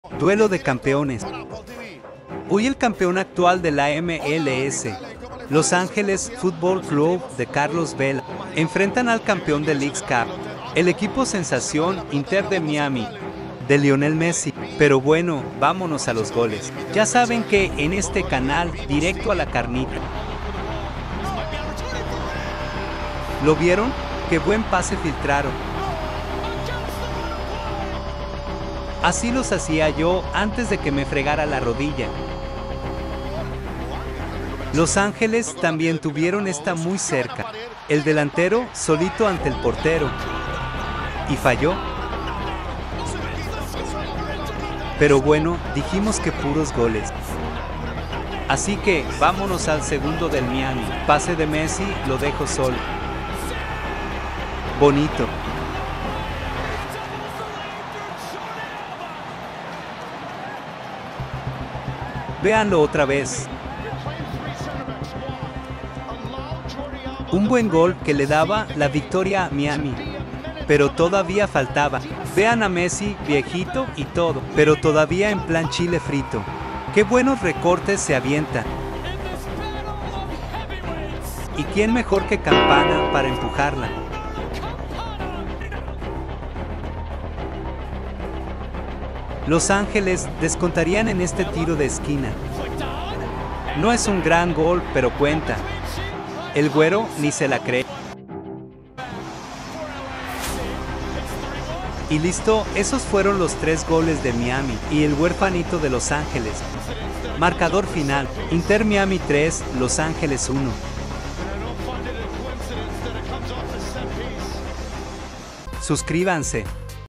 Messi contra Carlos Vela | Inter de Miami contra LAFC | Miami 3 - Los Angeles 1, todos los goles
Video resumen de todos los goles anotados en el encuentro de futbol soccer, jugado entre el Inter de Miami del argentino Lionel Messi y los angeles futbol club o LAFC, equipo del mexicano Carlos Vela, juego en el que gano Miami por 3 goles contra 1